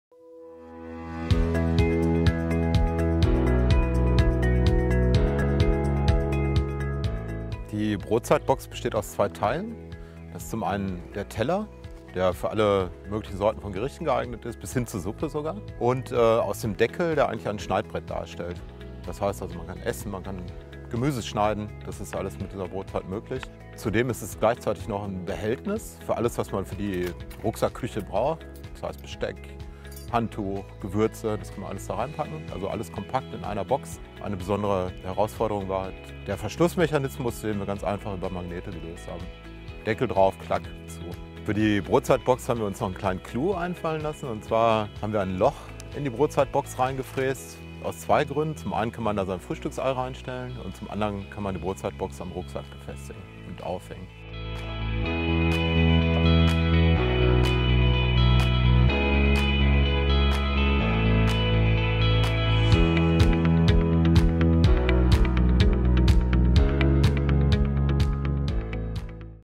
Brotzeit - Ordnung in der Rucksack-Küche by free*spee
Deshalb haben wir Brotzeit kreiert.
Aus massivem Holz, ehrlich und schön.
Jedes Brett ein Unikat.
Du brauchst unterwegs ein Brettchen,
um Gemüse zu schneiden oder Brote
zu schmieren? Aber auch einen Teller
für Nudeln oder Salat?. Brotzeit ist alles
in Einem.
Und der Clou: Der Deckel aus Birkensperrholz
hält dank starker Neodym-Magnete
sauber auf dem Unterteil. So wird Brotzeit
zur Butterbrotdose in der auch das
Besteck immer einen idealen Platz im
Rucksack hat.

Brotzeit ist...
• Teller und Brettchen in Einem

• eine magnetisch verschließbare Box

• Ordnung in der Rucksack-Küche:
Besteck, Gewürze & Co - alles gut verstaut

• eine Butterbrotdose mit Stil